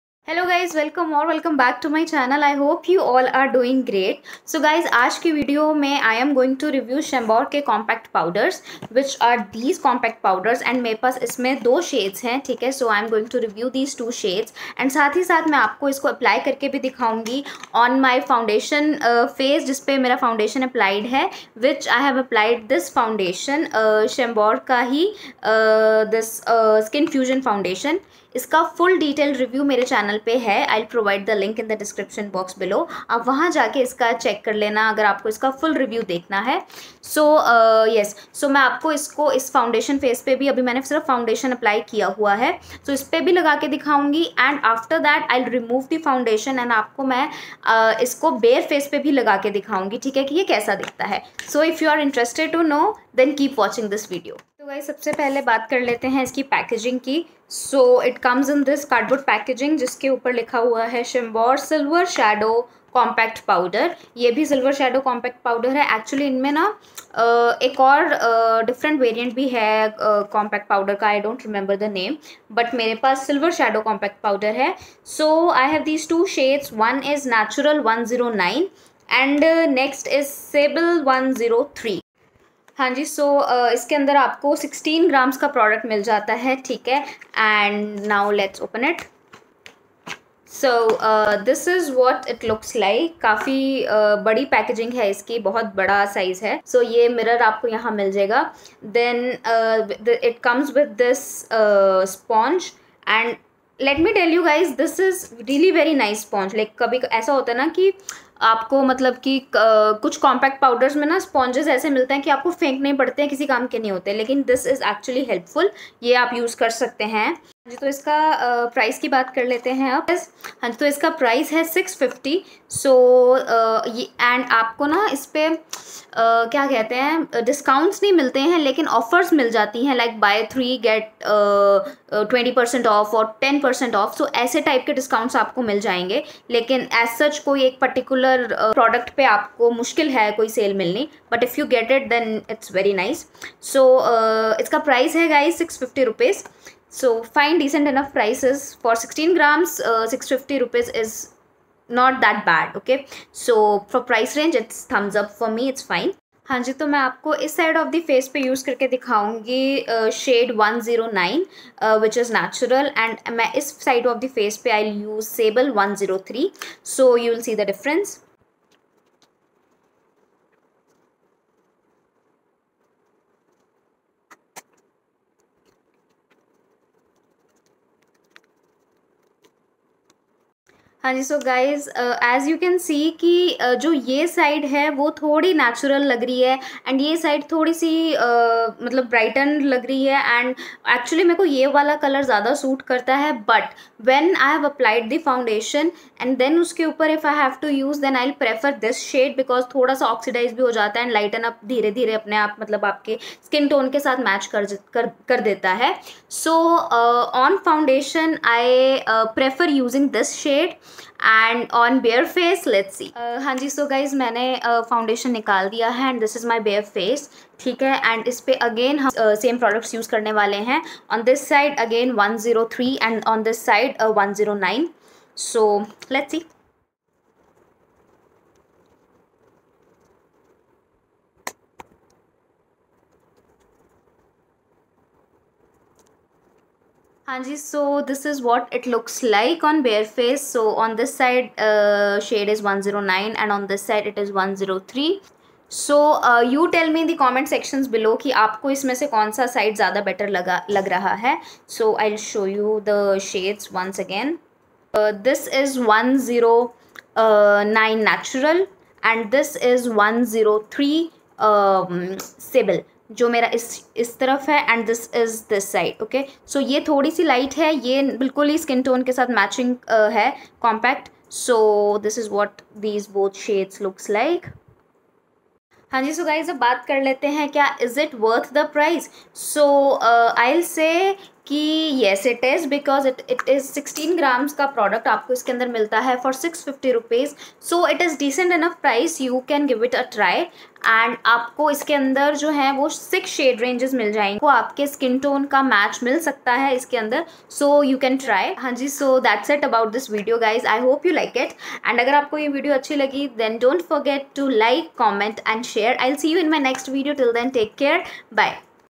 Honest review on CHAMBOR silver shadow compact powder | Natural 103 and Sable 109 || Review+Demo
I hope you find your perfect shade💗